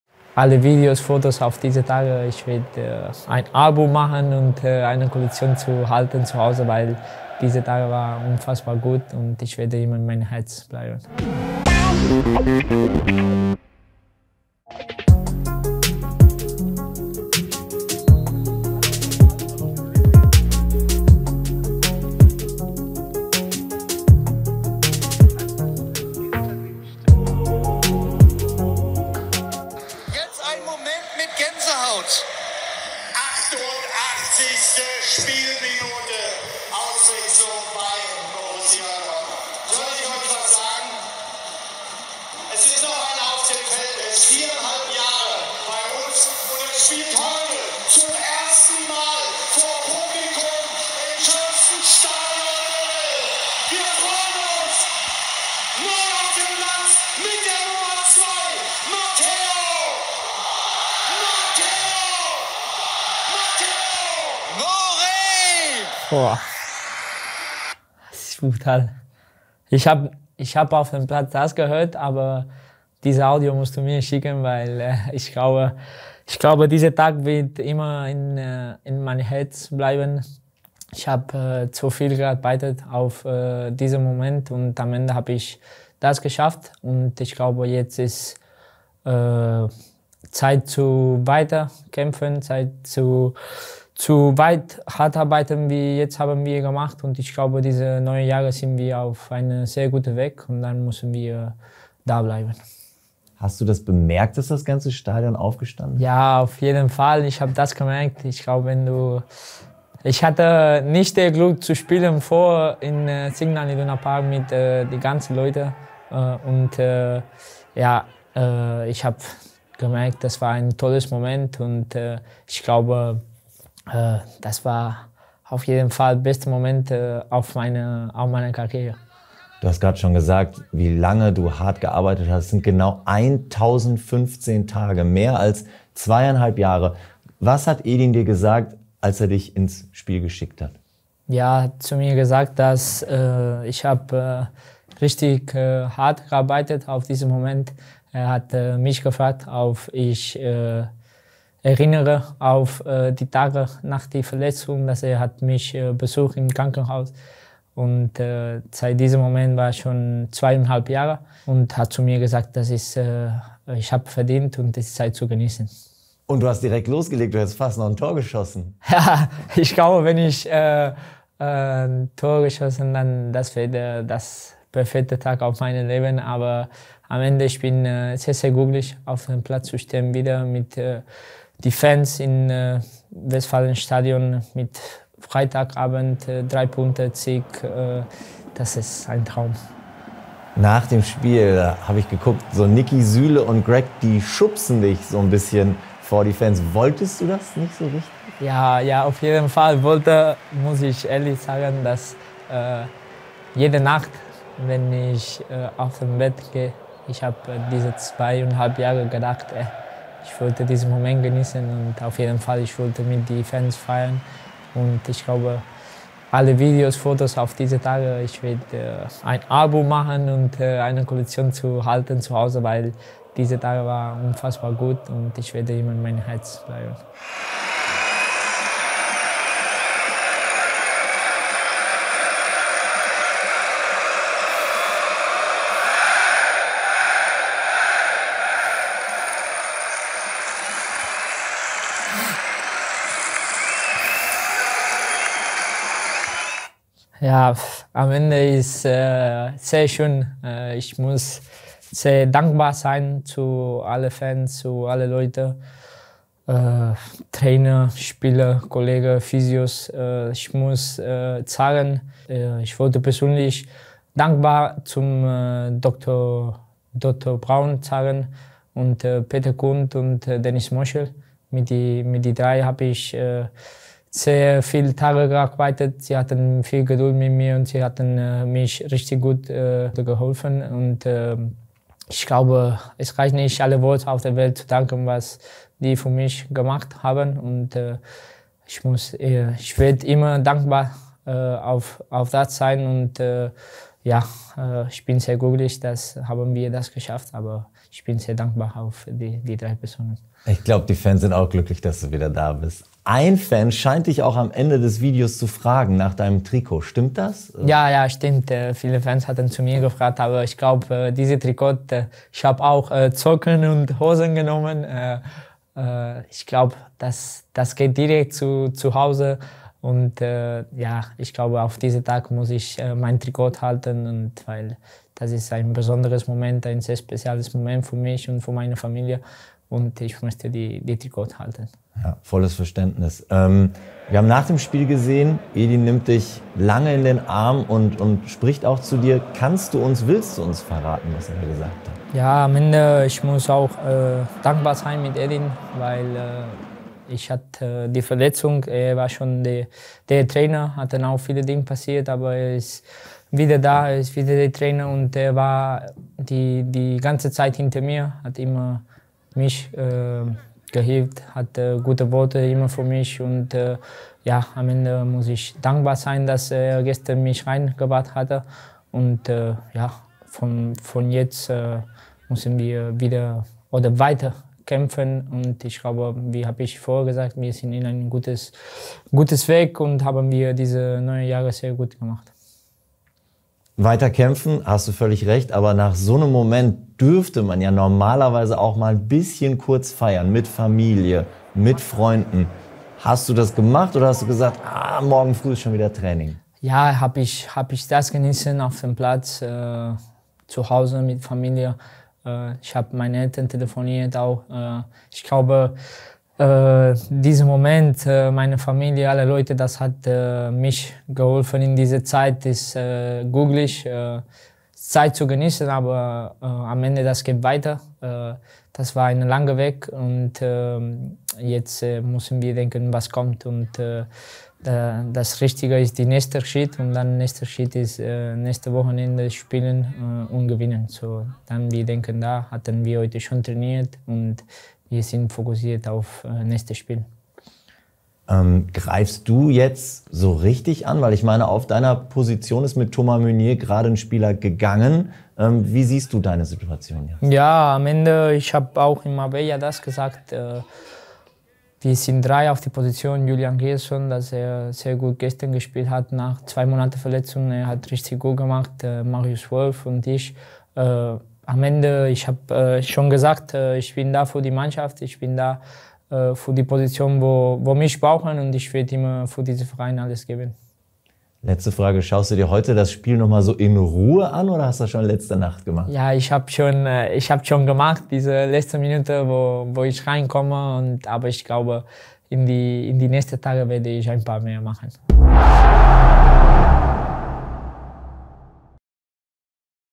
"This day will always stay in my heart!" | Interview with Mateu Morey
Mateu Morey has spent 1015 long days working his way back to the Bundesliga, back to SIGNAL IDUNA PARK. Yesterday, he took to the pitch for the first time in front of 81,365 fans. What was it like for the young right-back? In our interview with Mateu Morey, you can find out who he would particularly like to thank in retrospect, what Edin told him along the way and which Netradio clip gave him goosebumps.